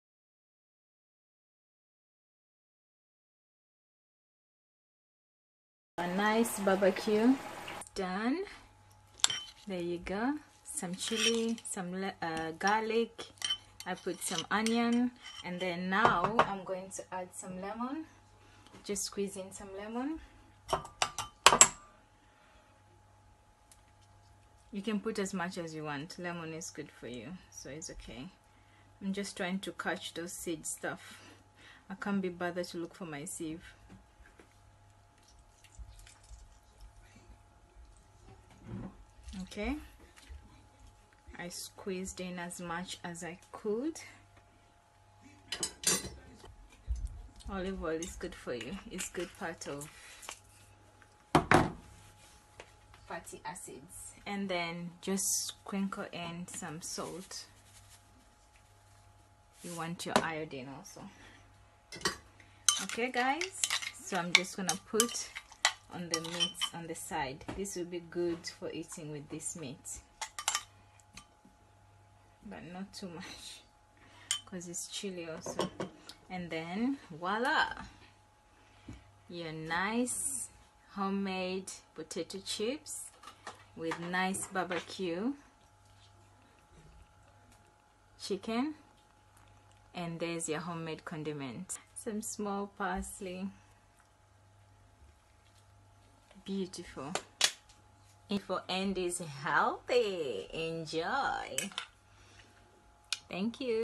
Potato Fries
Delicious quick and easy home made sweet potatoe fries served with bbq oven baked chicken and onion chilli condiment mmhhh yummy.
I cut/chopped potatoes in advance.
Sorry about the noise when I was looking for mu chopper tool :).
Correction point( I said meat meant chicken :).

Note: I do buy ready made potatoes many times from supermarket but also like to make them at home from scatch when I can .

Enjoy.